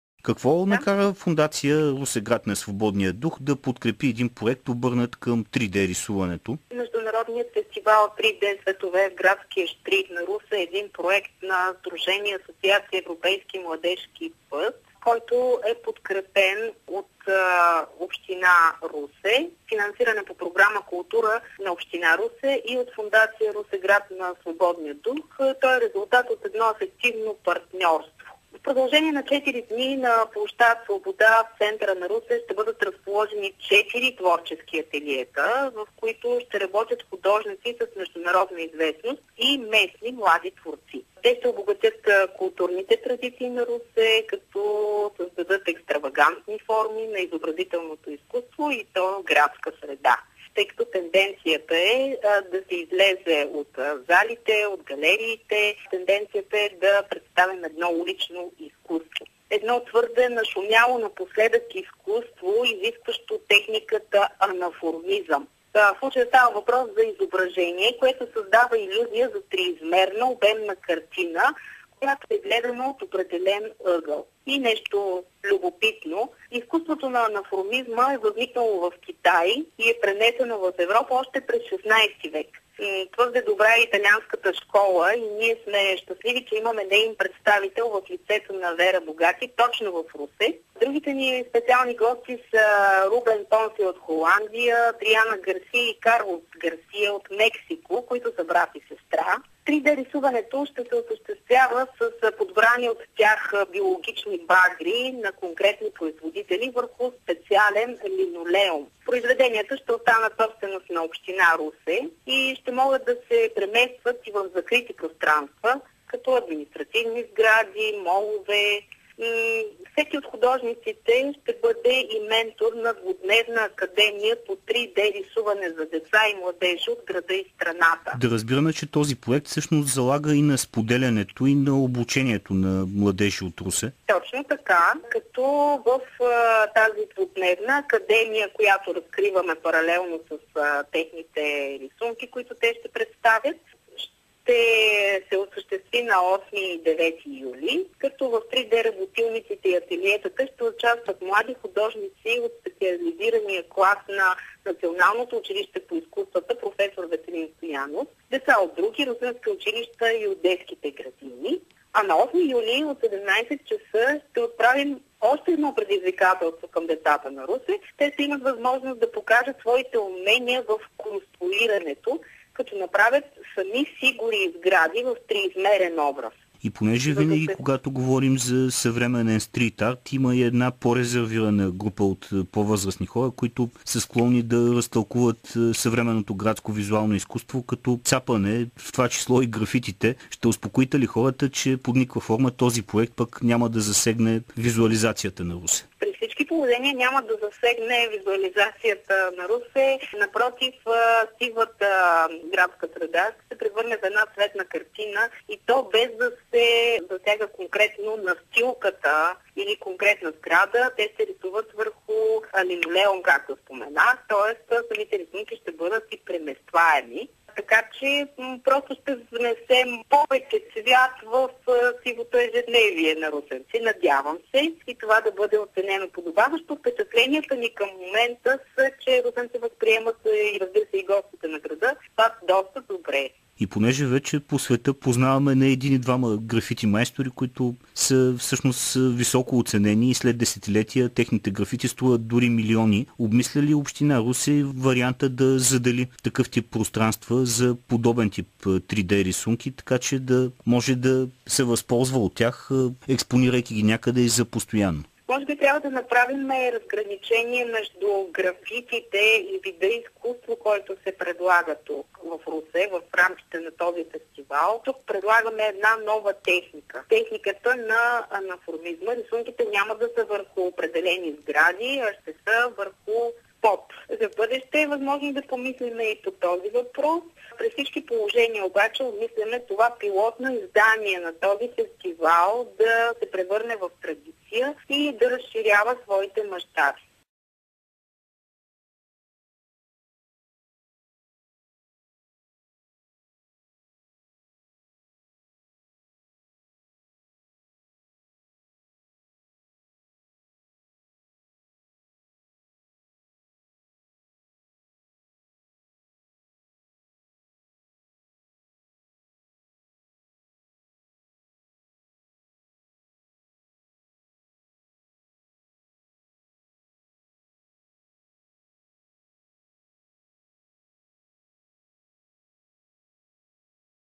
С Елена Минкова за „3D-светове в градския щрих на Русе“
Младежки проект в Русе от днес до неделя ще представи 3D рисуването. Той се изпълнява от Сдружение „Асоциация Европейски младежки път. В него участват художници от различни страни, със значим опит в изкуството. Още за артистичната проява, която се състои за първи път, чуйте от разговора на Антоний Димов с Елена Минкова, изпълнителен директор на Общинска фондация „Русе – град на свободния дух“. Записът е направен от ефирното излъчване на Радио Шумен, РРС на БНР.